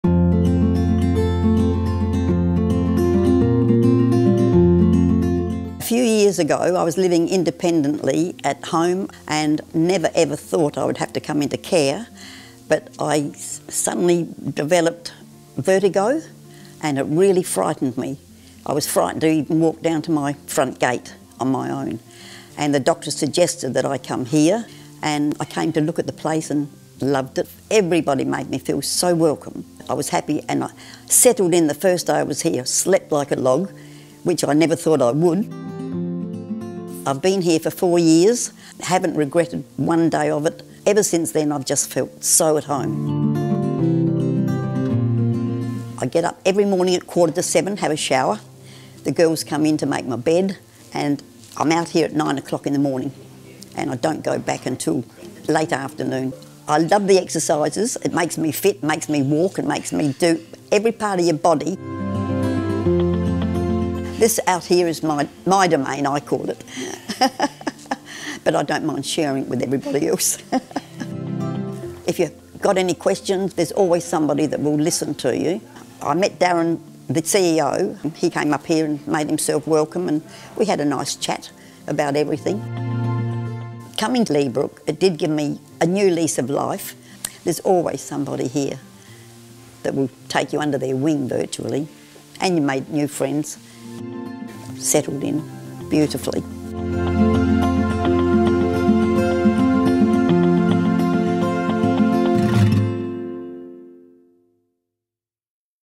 Resthaven Leabrook resident, Beryl — Testimonial
Resthaven Leabrook resident, Beryl, shares her experience.